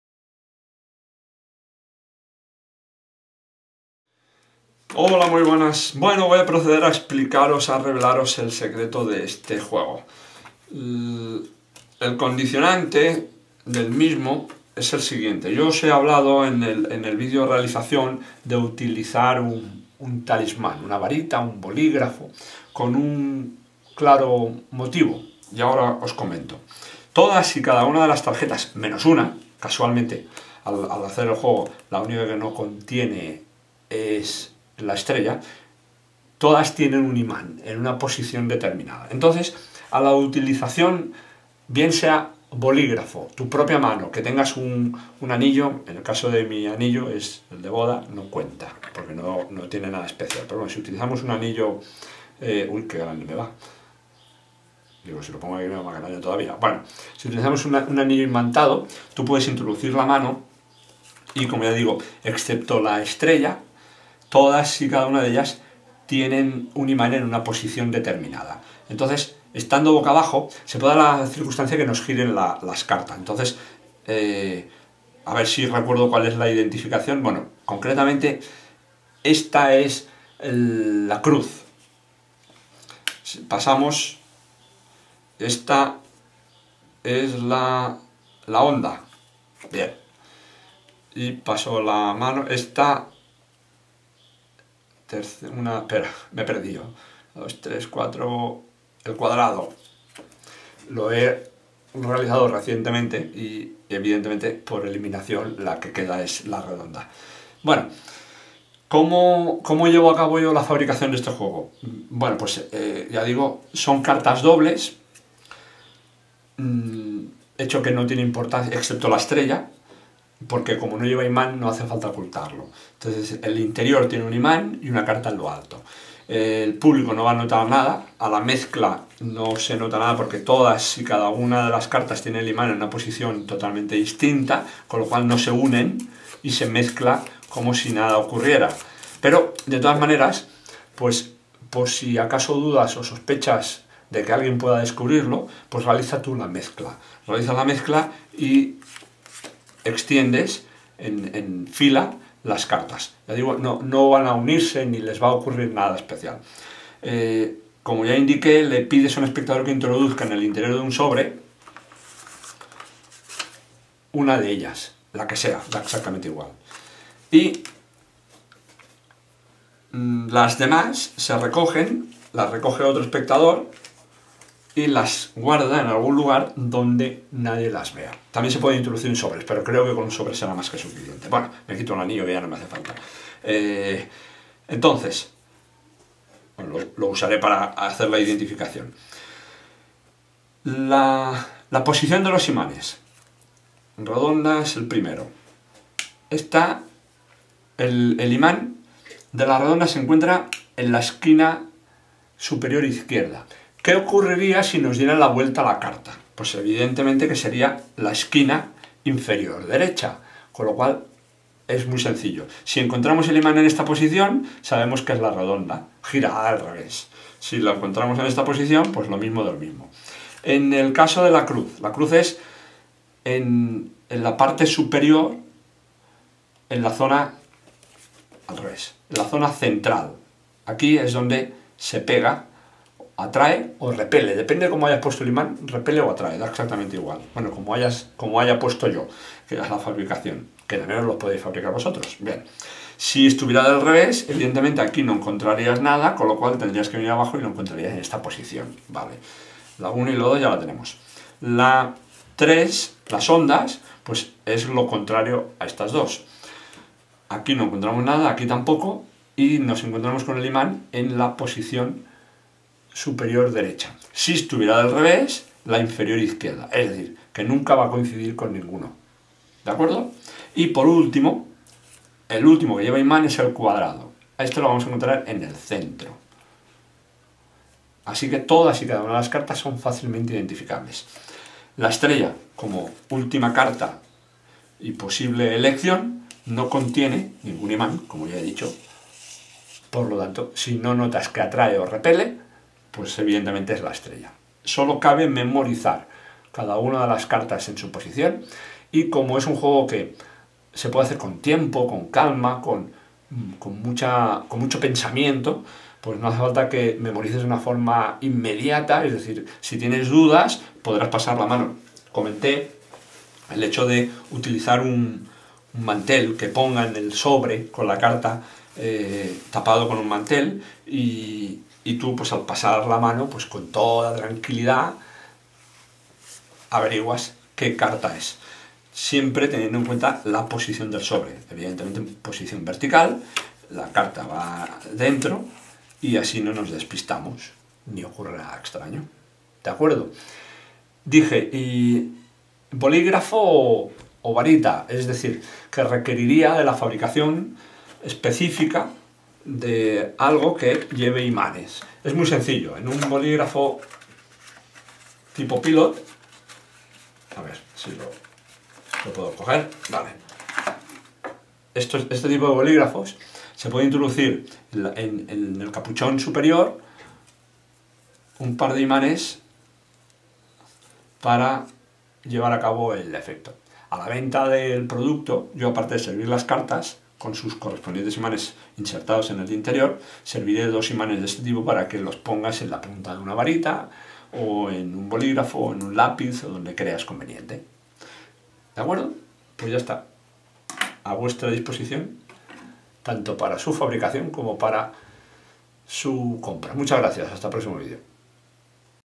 Truco de magia revelado - Increible ESP #106
Este vídeo corresponde al truco de magia no revelado núm. 100. Aquí lo encontrarás revelado para saber como hacerlo.

MagicUrzay